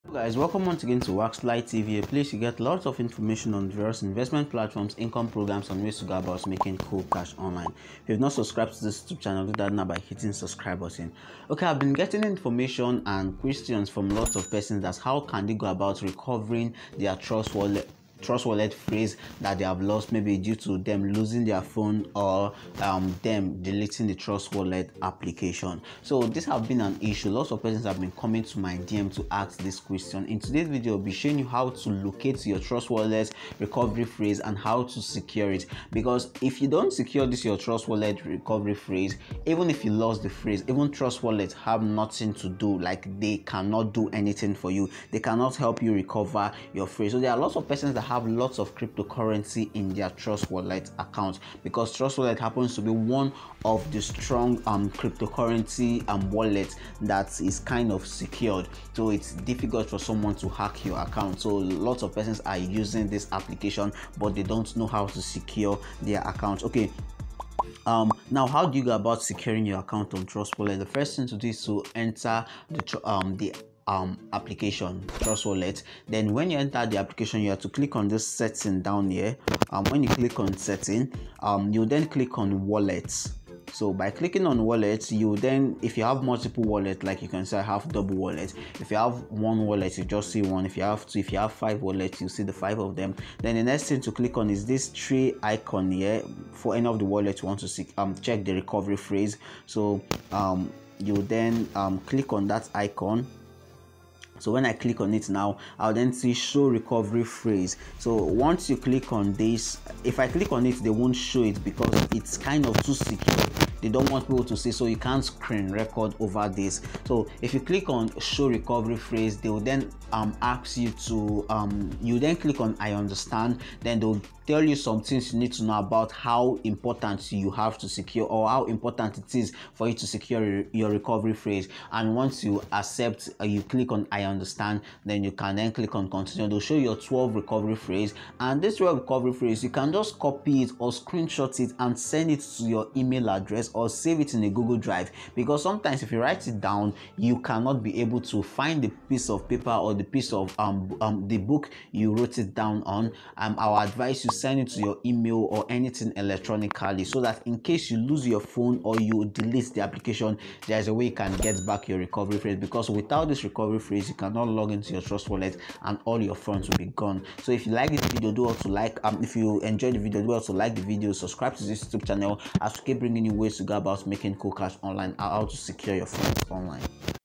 How to Locate 12 Word Recovery Phrase on Trust Wallet || Trust Wallet Hack for all Crypto Lovers.
You need to know how to Locate and Secure your Trust Wallet Phrase else if lost, your account can't be gotten back.

You can join us on WhatsApp also.

daily to get lots of information on ways you can make money online.